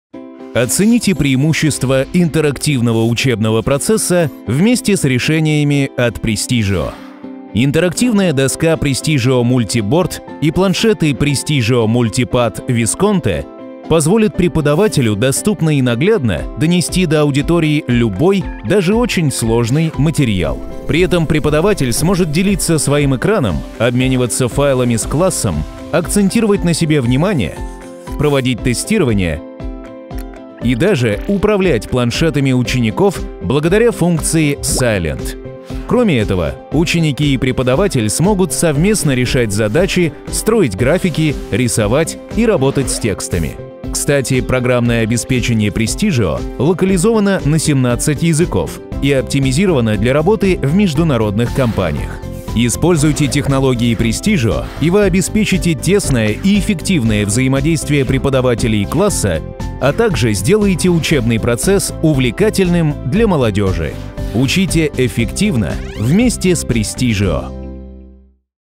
Prestigio MultiBoard - Solutions for Education (In Russian)
Оцените преимущества интерактивного учебного процесса вместе с решениями от Престижио.
Интерактивная доска Prestigio MultiBoard и планшеты Prestigio MultiPad Visconte позволят преподавателю доступно и наглядно донести до аудитории любой, даже очень сложный материал.
При этом преподаватель сможет делиться своим экраном, обмениваться файлами с классом, акцентировать на себе внимание, проводить тестирование и даже управлять планшетами учеников благодаря функции "Сайлент"
Кроме этого ученики и преподаватель смогут совместно решать задачи, строить графики, рисовать и работать с текстами.
Кстати, программное обеспечение Prestigio локализовано на 17 языков и оптимизировано для работы в международных компаниях.
Используйте технологии Prestigio и вы  обеспечите тесное и эффективное взаимодействие преподавателя и класса, а также сделаете учебный процесс увлекательным для молодежи
Учите эффективно вместе с Prestigio!